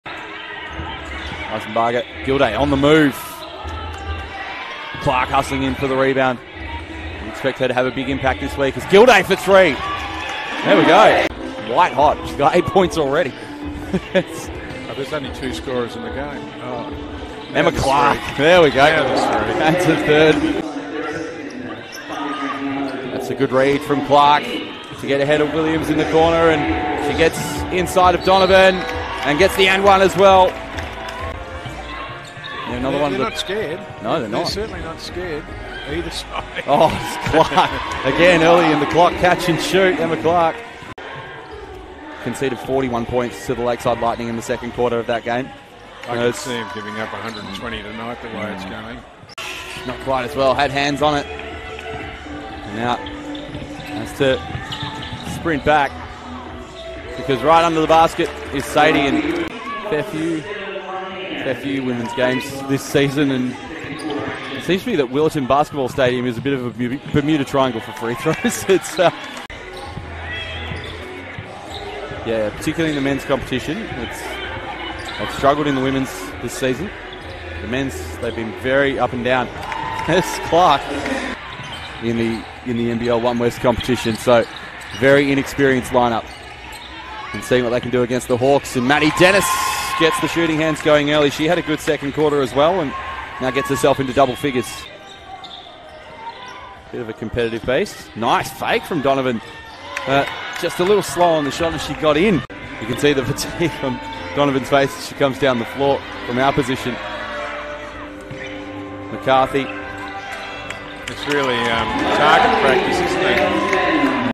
NBL1 action: Perry Lake's Emma Clarke vs South-West Slammers
After being sidelined for one week due to an illness, Sydney Flames wing Emma Clarke shone with 18 points, 11 boards, four assists, three steals and one block at Perry Lakes belted the South-West Slammers 102-47.